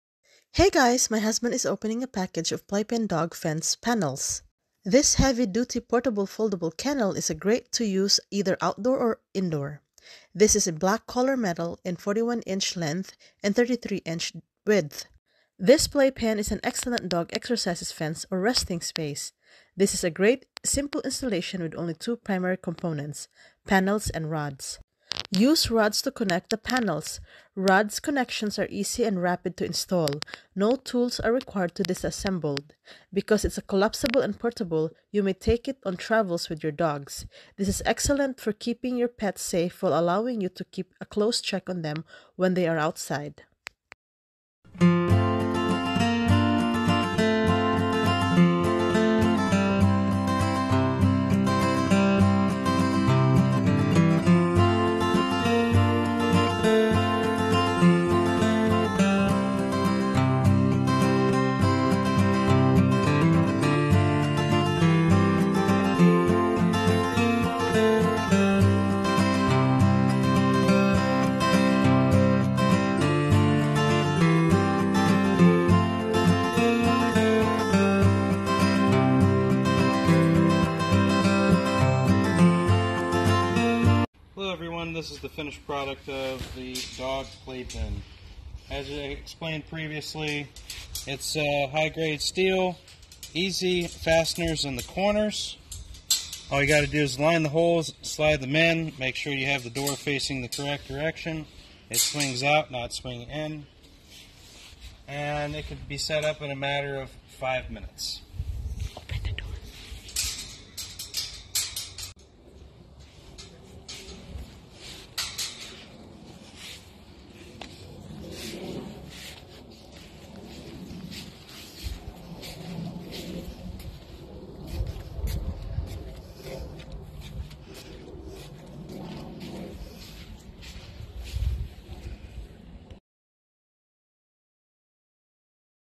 Dog Playpen Kennel Video Review - Amazon Product
This metal dog playpen fence is suitable for use as an exercise fence, a playpen, or a resting place. In a pet-friendly area, there is enough of space for pets to enjoy exercise and other outdoor activities.

Will create a shoppable video for your products at a reasonable price!!!